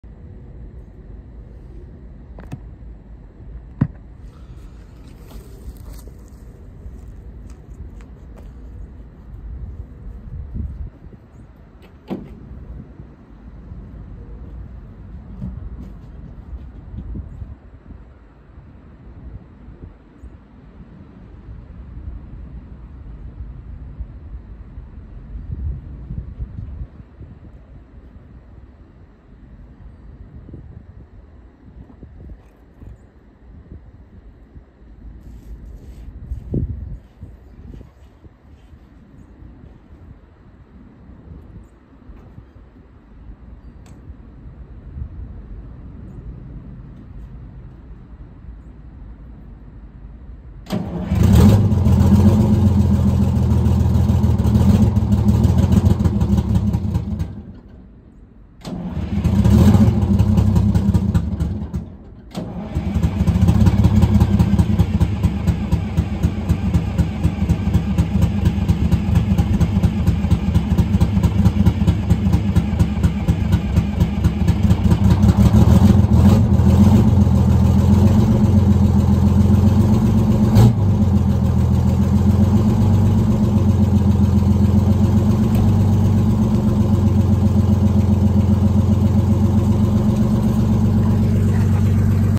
671 Detroit cold start. GMC 9500.
1972 GMC 9500 cold start. 671. 50 degrees. First start since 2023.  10 speed road ranger.  238 Detroit Diesel.  Maine